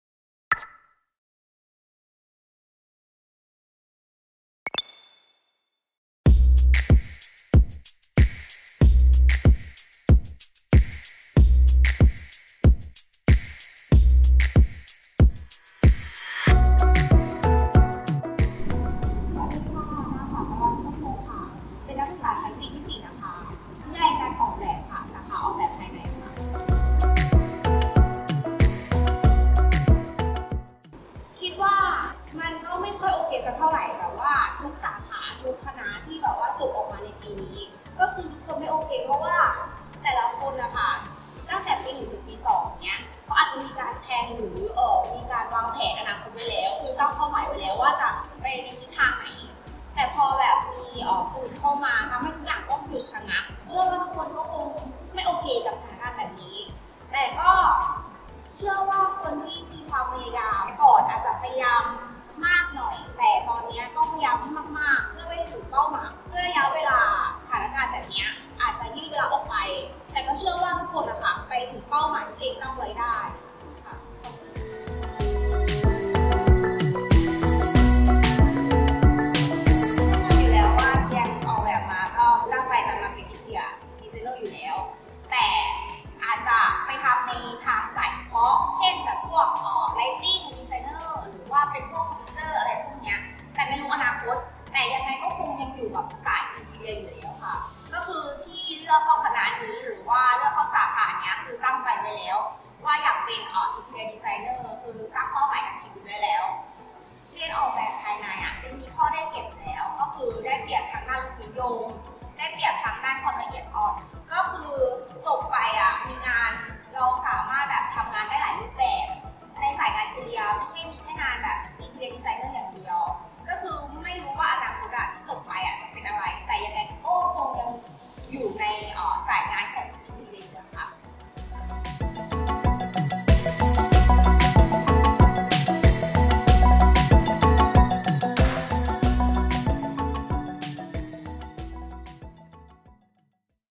GRADUATE TALKS !
'EPISTEMOLOGY I The Theory of Knowledge'

เเนวคิดสร้างสรรค์ดีๆ จาก เจน บัณฑิตใหม่ของสาขาออกเเบบภายใน ... เเละอย่าลืมติดตามนิทรรศการโครงการศิลปนิพนธ์ประจำปีรูปเเบบ On-Ground to Online เเคมเปญชื่อ 'GRAB AND GO 2021 - EPISTEMOLOGY' ได้ที่ Academic Pavilion ส่วนหนึ่งของ Bangkok Design Week 2021 ภายในอาคาร สามย่าน มิตรทาวน์ เเละทางออนไลน์ที่

Grab and Go 2021 Thesis Art Exhibition will be showcase at Samyan Mitrtown Hall, 5th floor from July 3th-11th, 2021 or simply scan the QR code to virtually unveiled the exhibition.

FB GRAB and GO : Art Thesis Exhibition
IG rsuinteriordesign
Youtube Interior Design RSU